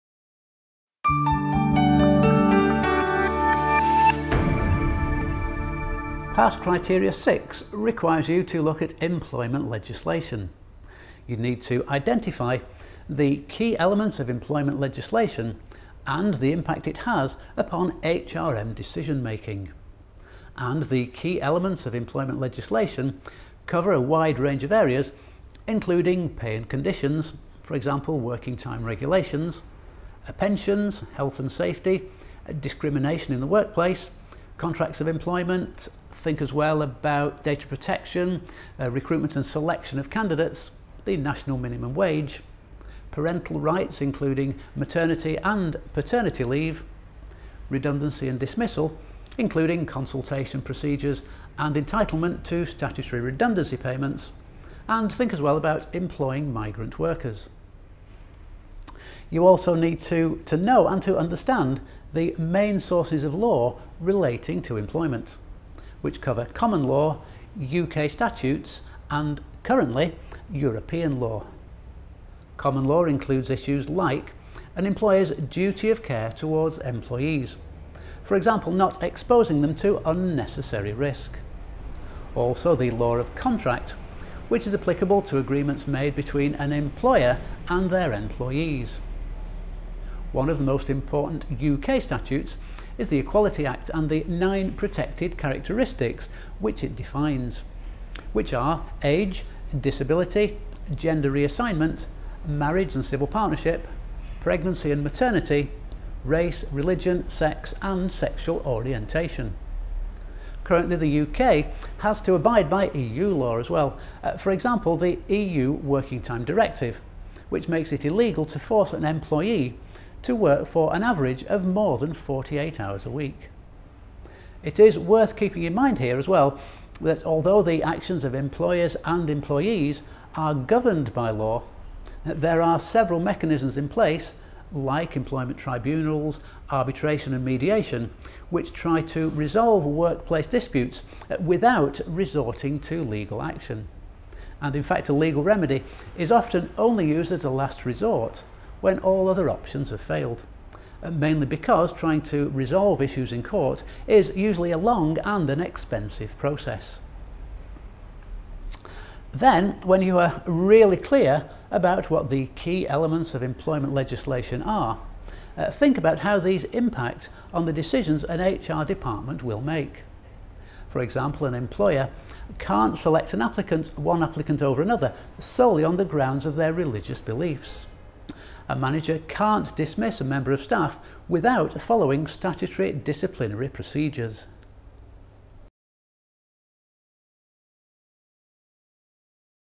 BTEC HND - Business Unit 3, P6
Pearson BTEC Business, Unit 3 Human Resource Management. A guideline on how to Pass (Criteria 6) in assignment work.